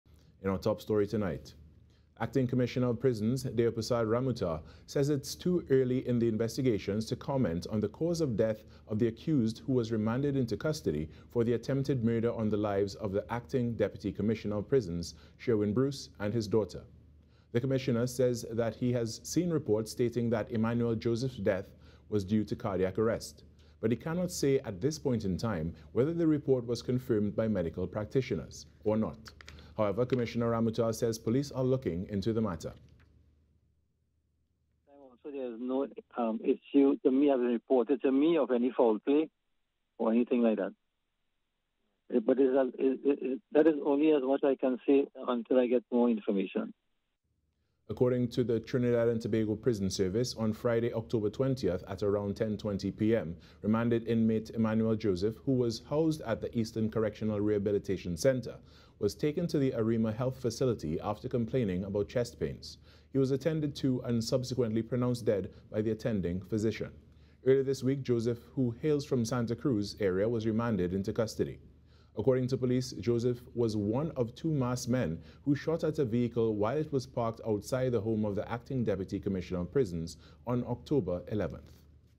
Police Investigating Emmanuel Joseph's Death
Acting Commissioner of Prisons, Deopersad Ramoutar says its too early in the investigations to comment on the cause of death of the accused who was remanded into custody for the attempted murder on the lives of the acting deputy commissioner of prisons, Sherwin Bruce and his daughter.